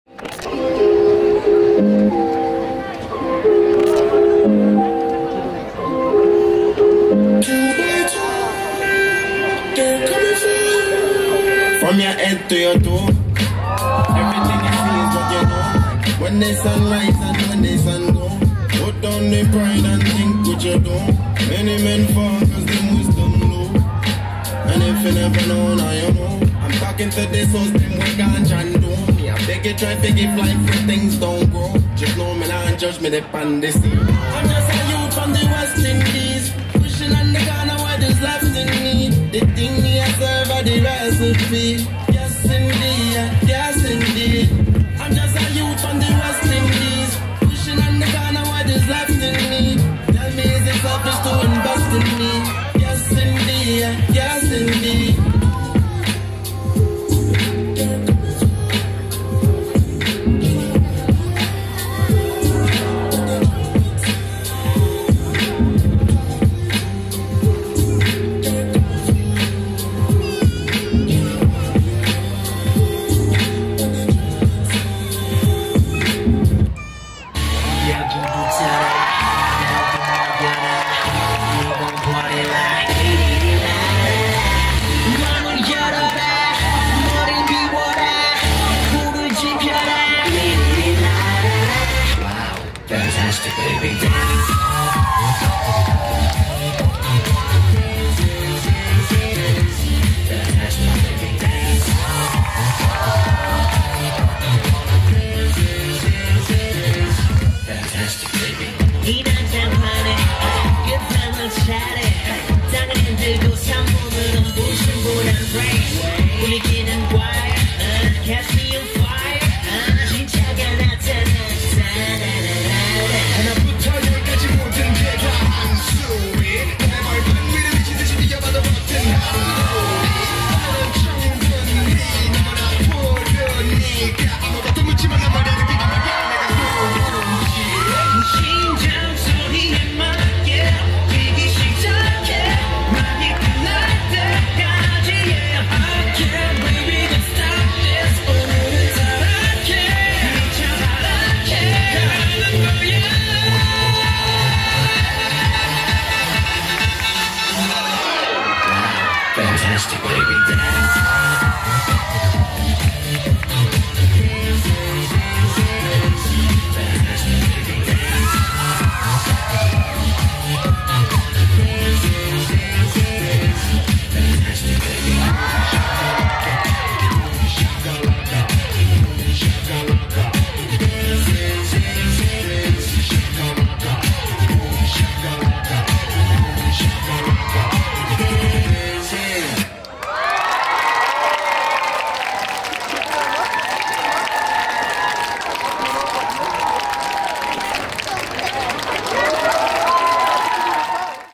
B-On Top Vol. 31 / Guest Showcase / H.L ROCKERZ / 2022.08.27.
Korea Best Monthly Breaking Battle Event "B-On Top" official Channel

B-On Top Vol.31 (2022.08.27 Sat)
ㅤㅤ
Judge
- NOODLE (GAMBLERZ CREW)
ㅤㅤ
- FE (FLOWXL)
ㅤㅤ
- SHINOBI (RIVERS CREW)

Dj
- BREAKSON
ㅤㅤ
Mc
- B.D (A-Run Company)
ㅤㅤ
Guest Showcase
- H.L ROCKERZ

- Info -